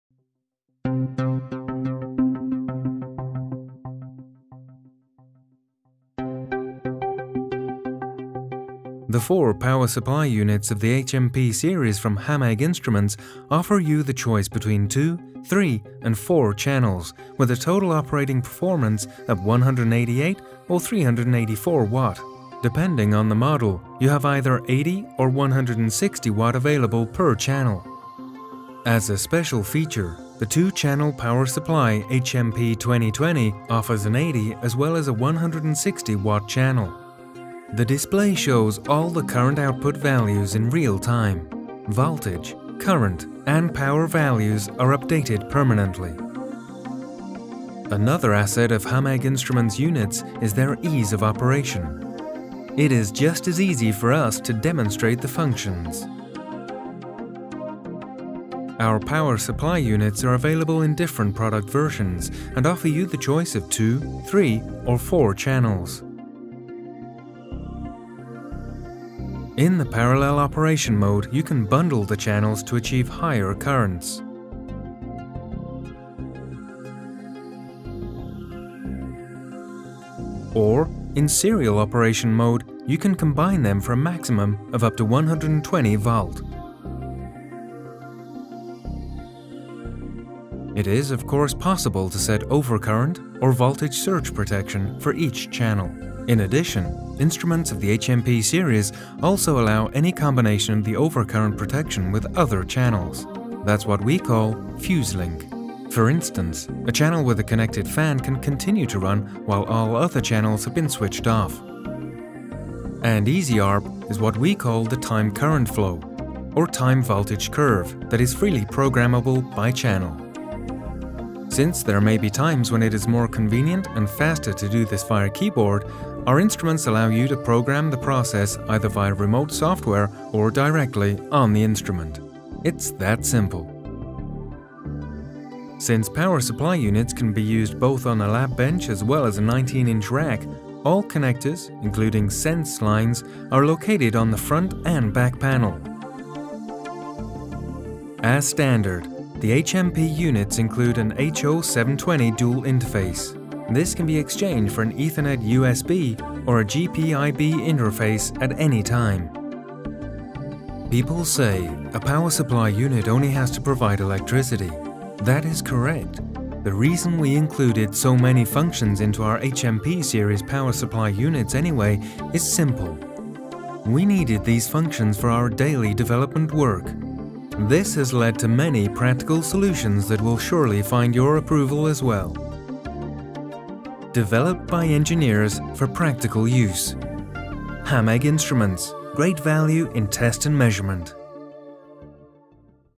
The Hameg Power Supplies HMP Series
The four power supply units HMP 2020, HMP 2030, HMP 4030 and HMP 4040 from HAMEG Instruments offer you the choice between 2, 3 and 4 channels with a total output power of 188 or 384 watt. Depending on the model, you have either 80 or 160 watt available per channel. The display shows all the current output values ​​in real time. Voltage, current and power values are updated permanently